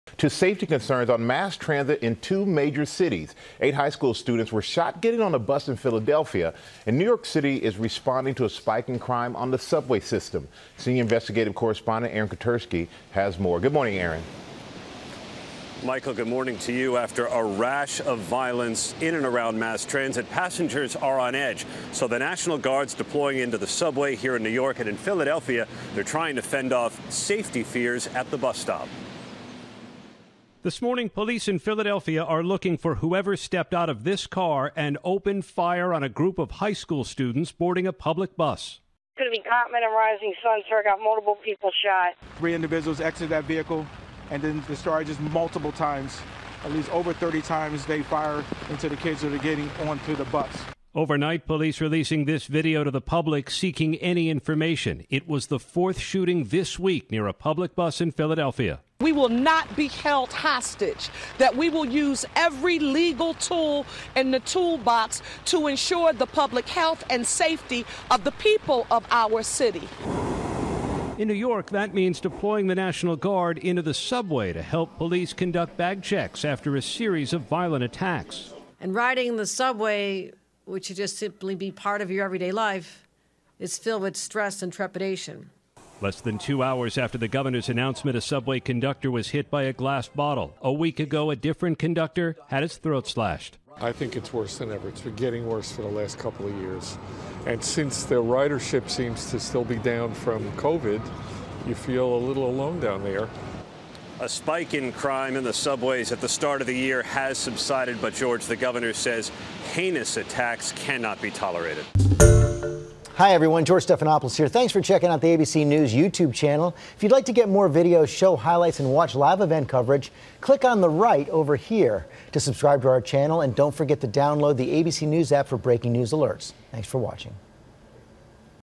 ABC News Alert: "Mass shooting at Philadelphia bus stop"
Concern for safety on mass transit is on the rise after eight students were shot at a bus stop in Philadelphia.